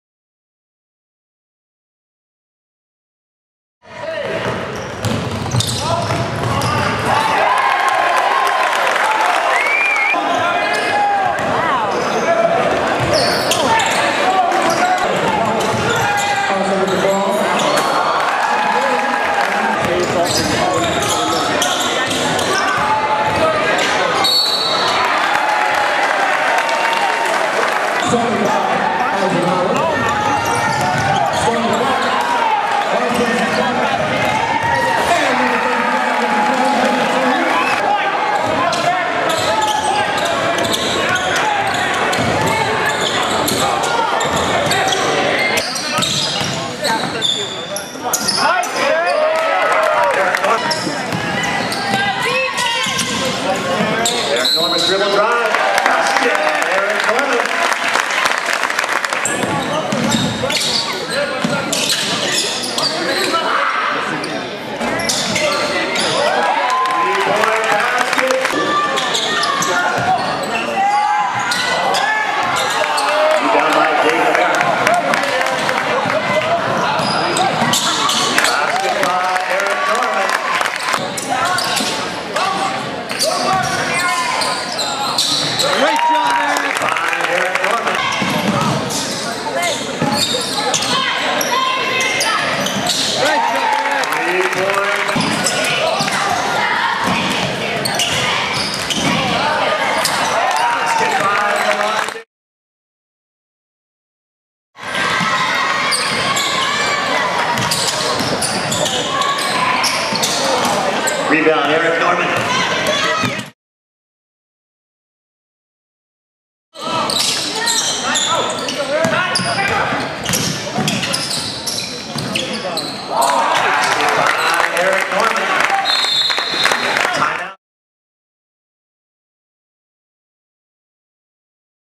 Eric Norman '11, Parker Senior Year
A tall 6' 9" forward on a very tall Parker team Eric Norman was the MVP for the North in the senior all-star game.   Norman is headed to SMU next year.

0:03  Dunk
0:10  Block/Steal
0:28  Dunk
0:38  Dribbling
1:28  3-Ball

Parker (San Diego, CA) def. Poway (CA) 63-46
2010 Hosea Hall Christmas Invitational 1st Round
December 18, 2010 at Lincoln (San Diego, CA)
Parker (San Diego, CA) def. Foothills Christian (El Cajon, CA) 71-43
January 7, 2011 at Parker (San Diego, CA)

Award:  2011 CIF-San Diego Section Second Team

College:  SMU